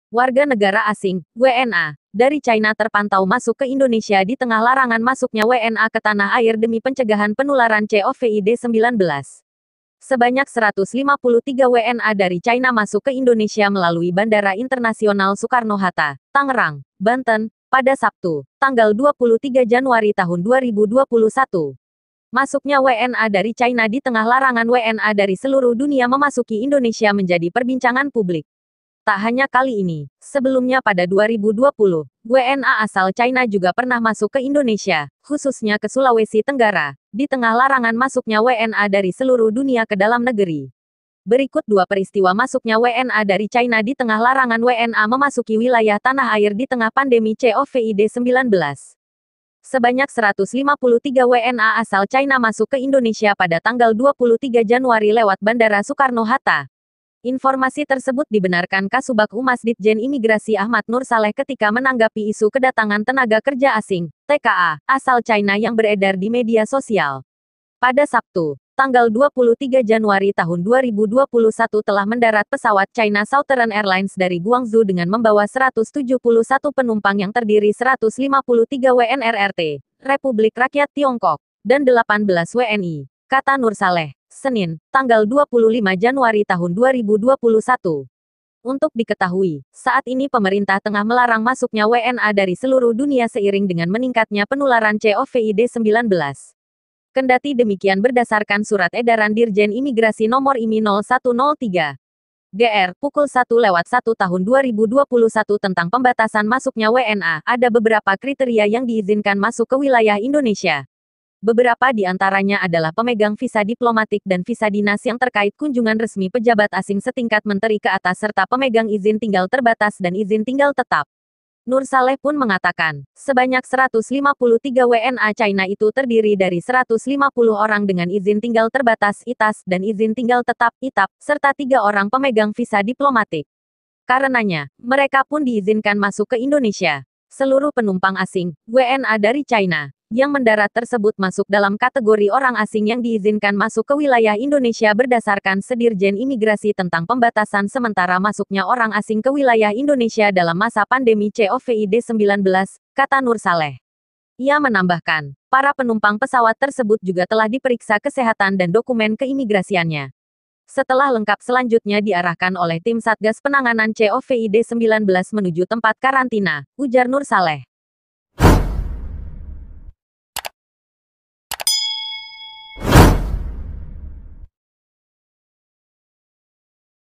153 Warga China Tiba di Indonesia di masa pandemi
Sebanyak 153 Warga Negara Asing (WNA) asal China tiba di Bandara Seokarno-Hatta, Banten, pada Sabtu 23 Januari 2021.

Meski sebenarnya larangan WNA masuk ke Indonesia masih berlaku, kedatangan 153 orang asal China ini telah dikonfirmasi langsung oleh Kasubag Humas Ditjen Imigrasi, Ahmad Nursaleh.